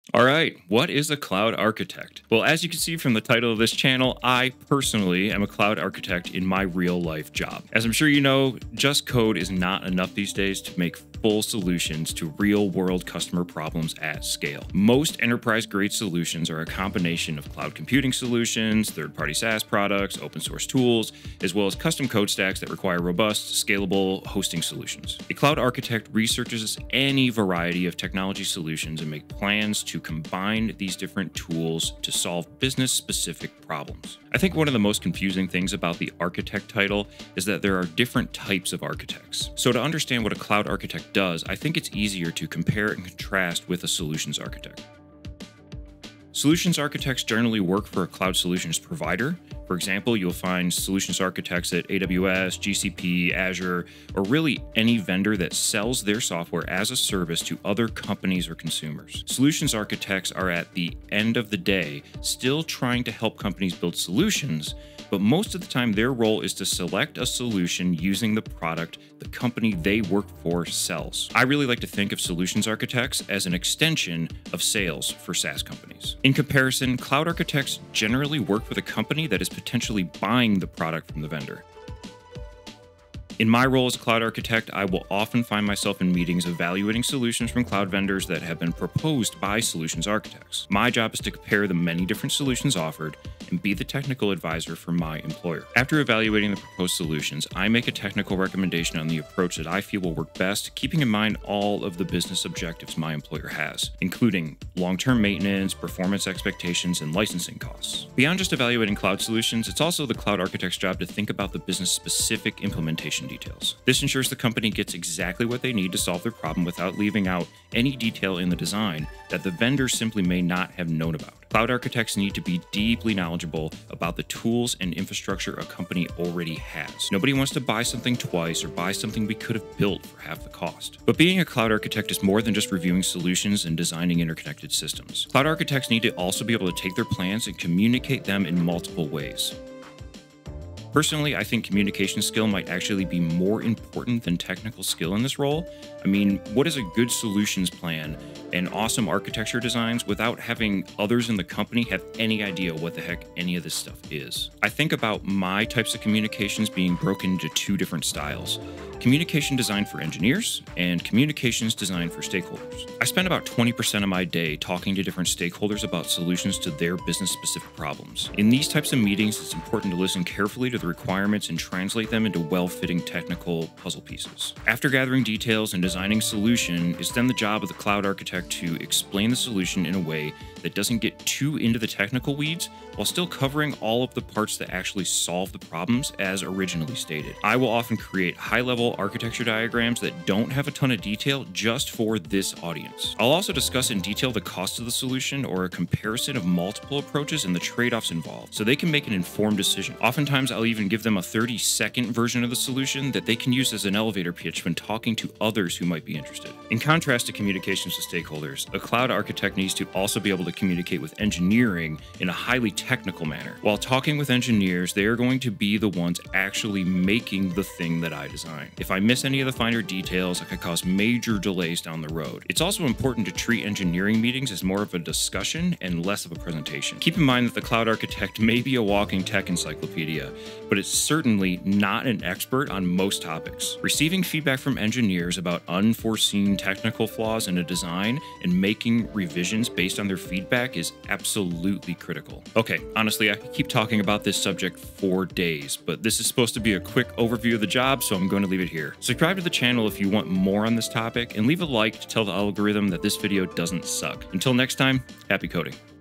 Unpacking the Cloud Architect Role: From Solutions to Code
As you can see from the title of this channel, I am personally a cloud architect in my “real life” job.

As im sure you know, just code is not enough these days to make full solutions to real world customer problems at scale. most enterprise grade solutions are a combination of cloud computing solutions, third party SAAS product, open source tools, as well as custom code stacks that require robust, scaleable hosting solutions. A cloud architect researches any variety of technology solutions and makes plans to combine these different tools to solve business specific problems.

I think one of the most confusing things about the “architect” title is that there are different types of architects. the two types of architects you hear about most often are cloud architects and solutions architects. so to understand what a cloud architect does, i think its easier to compare and contrast with a solutions architect.

solutions architects generally work for a cloud solutions provider. for example, you will find solutions architects at AWS, GCP, Azure or really any vendor that sells their software as a service to other companies or consumers. solutions architects are at the end of the day still trying to help companies build solutions, but most of the time their role is to suggest a solution using the product the company they work for sells. I really like to think of solutions architects as an extension of sales for SAAS companies.

in comparison, cloud architects generally work for the company that is potentially buying the product from that vendor. In my role as a cloud architect I will often find myself in meetings evaluating solutions from cloud vendors that have been proposed by solutions architects. my job is to compare the many different solutions offered and be the technical advisor for my employer.

being a company’s technical consultant

After evaluating the proposed solutions, i make a technical recommendation on the approach that i feel will work best, keeping in mind all of the business objectives my employer has. including long term maintenance, performance expectations and licensing costs.

communications

but being a cloud architect is more than just reviewing solutions and designing interconnected systems. Cloud architects need to also be able to take their plans and communicate them in multiple ways. Personally, i think communication skills might actually be more important than technical skills in this role. i mean, what good is an amazing plan and awesome architecture diagrams without having others in the company have any idea what the heck any of this stuff is?

If you want to keep up on the latest in tech and development-related news, Subscribe to the weekly cloud, my weekly email newsletter where I sift through the internet so you don’t have to.